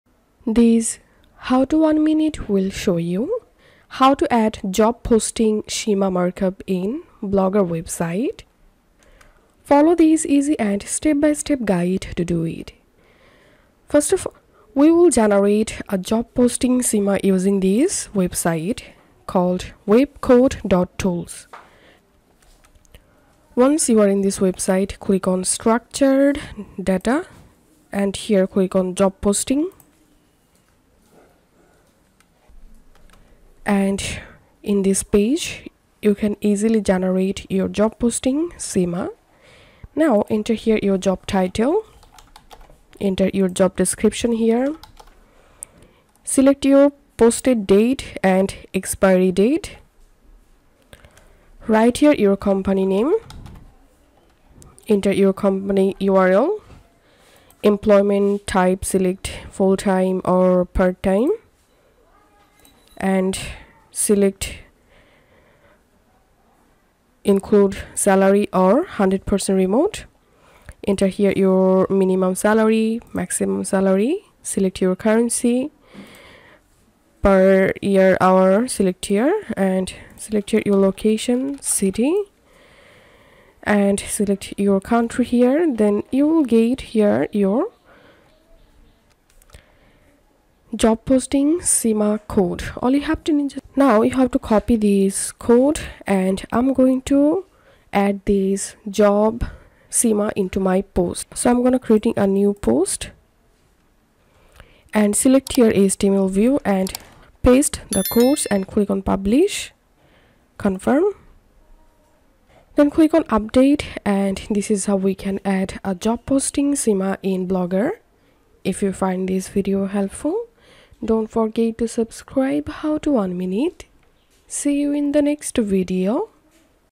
How to add Job Posting Schema Markup in Blogger/Blogspot 2026
Add schema markup to the Blogger blog and improve the SEO of the website. In this tutorial guide, add JSON-LD Schema.org Markup Structured Data.